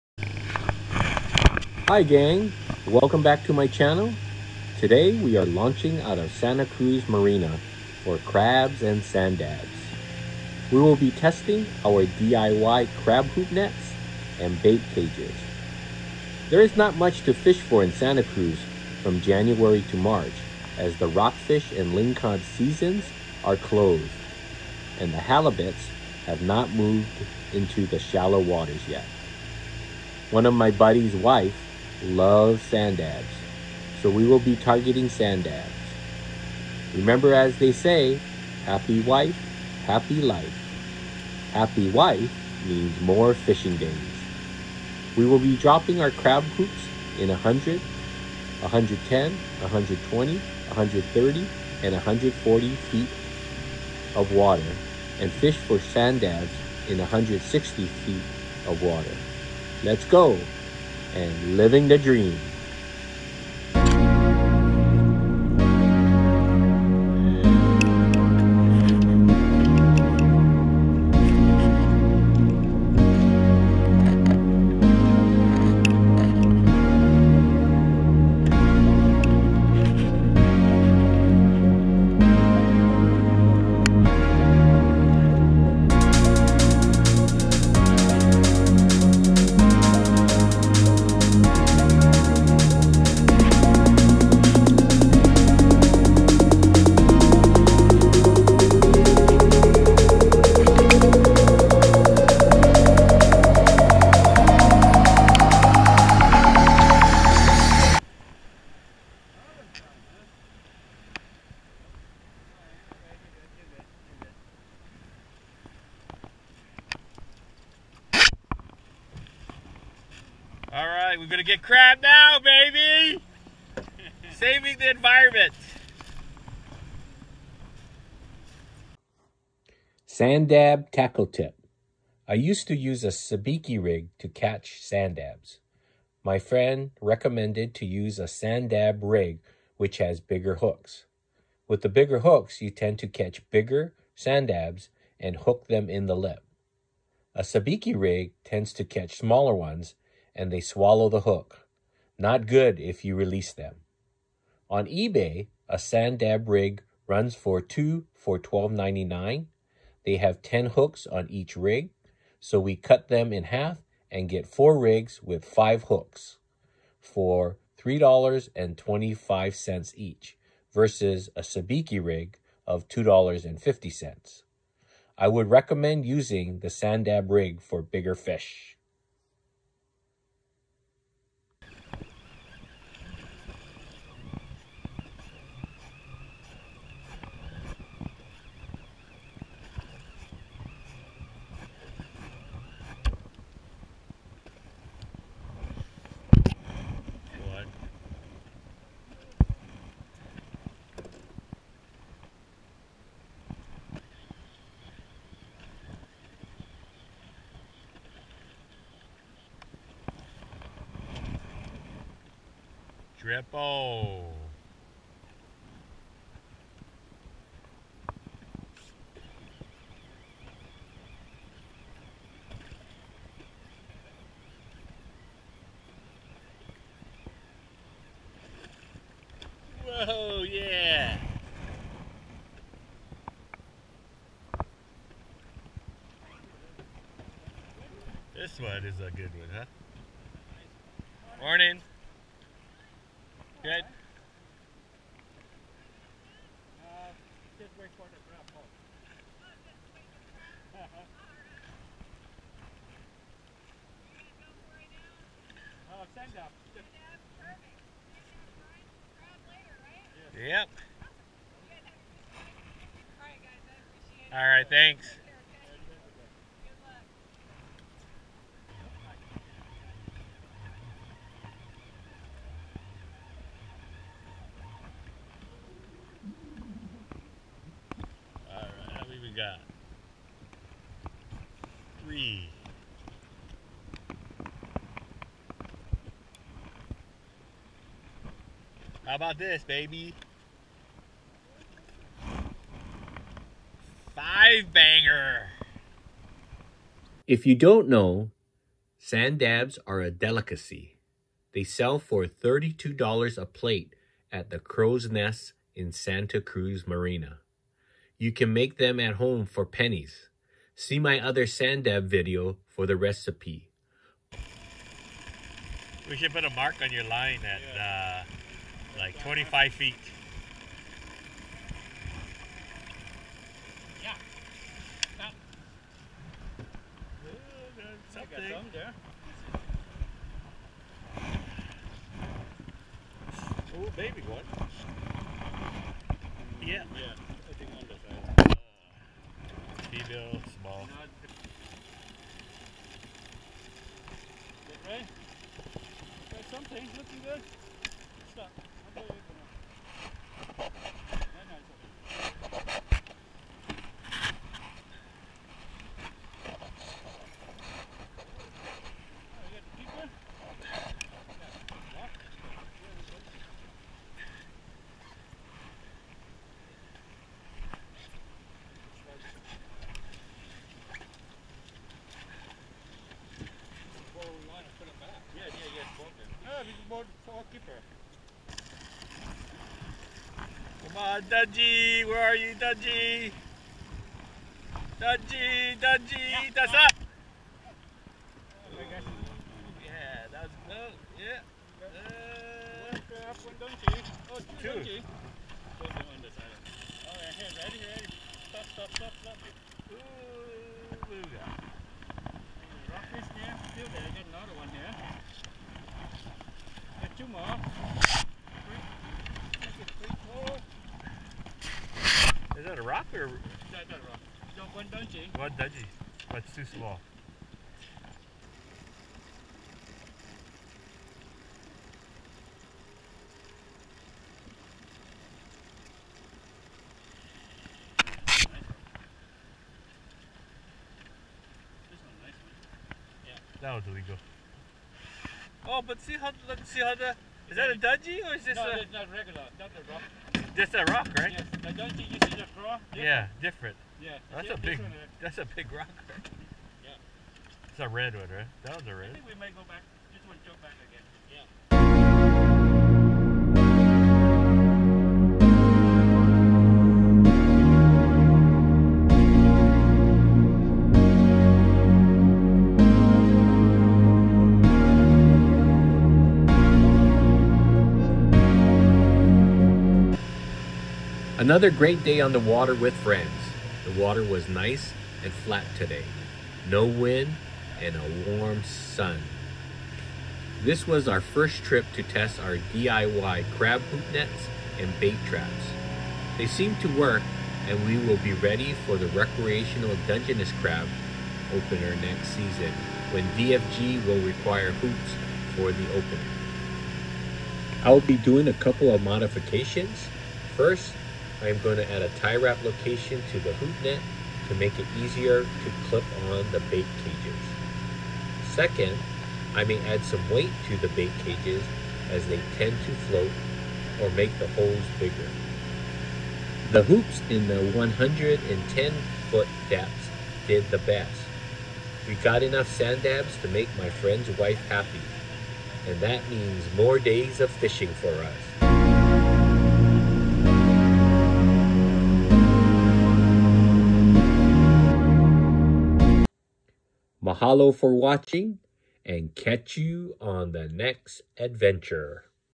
Santa Cruz: Testing our DIY crab hoop nets / bait cages and catching sanddabs for the wife.
With the rockfish and lingcod season closed to 3/31/22 and the halibuts are not yet in the shallow water in Santa Cruz, we decided to test our DIY crab hoop nets and bait cages and target sanddabs to past the time.